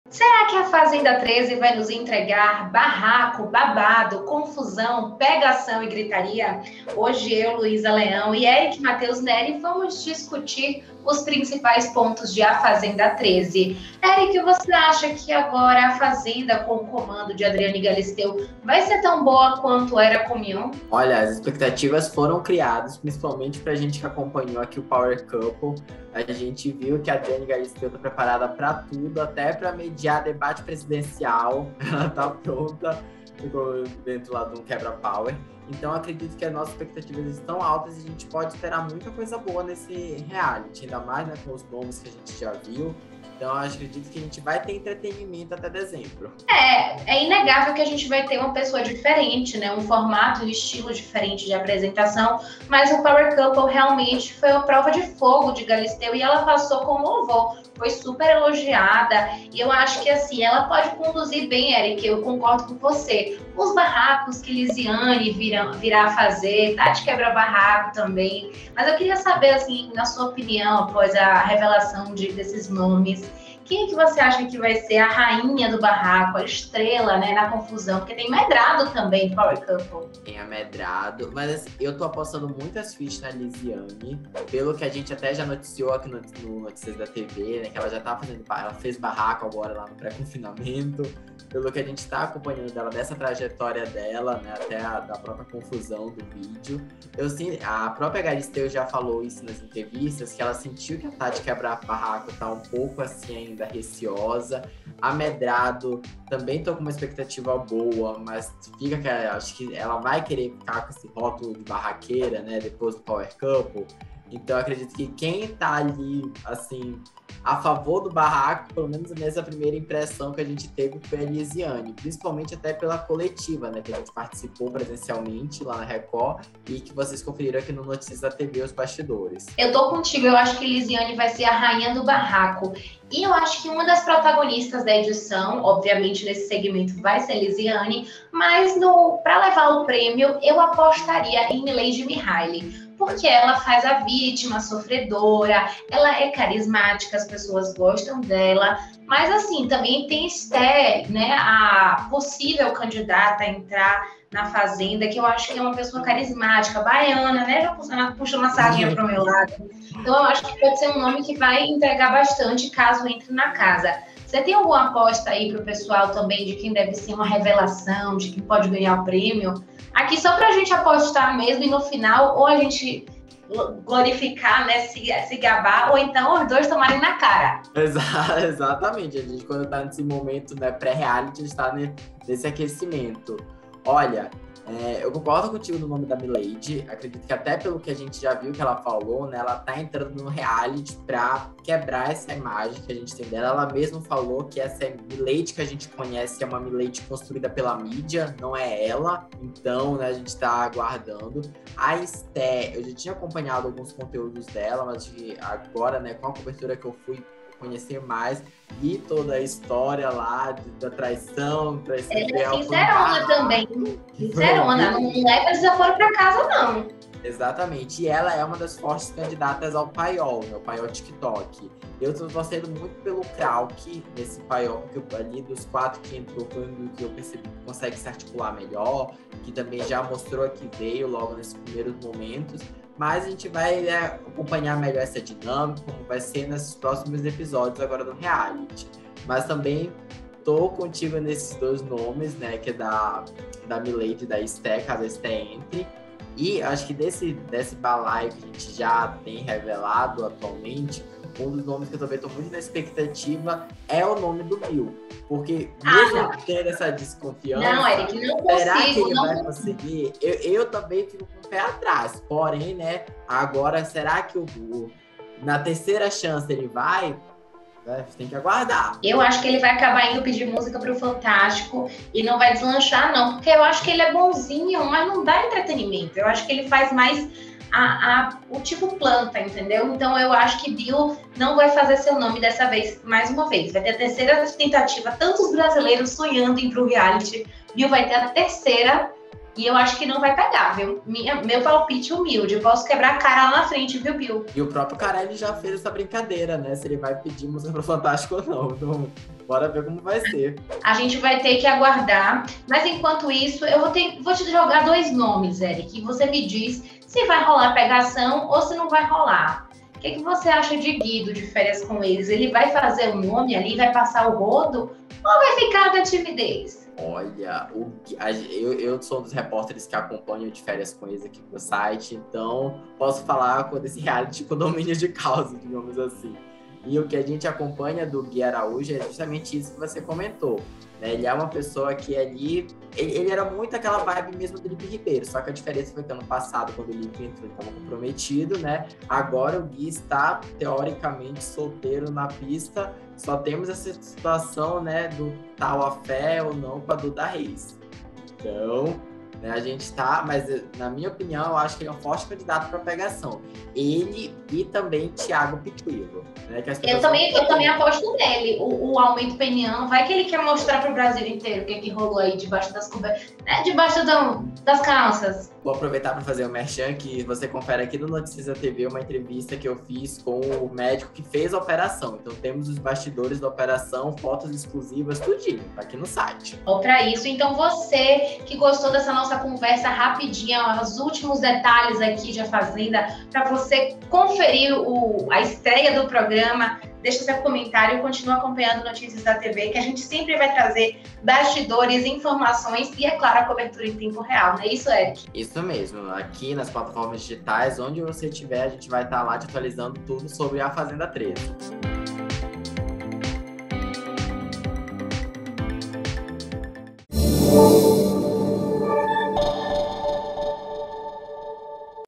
O que esperar de A Fazenda 13? Elenco promete barraco, pegação e gritaria
Agora sob o comando de Adriane Galisteu, A Fazenda 13 vai superar a última edição? O elenco escalado conta com barraqueiros de marca maior, doidos por pegação e históricos com polêmicas nada discretas. Saiba tudo no vídeo!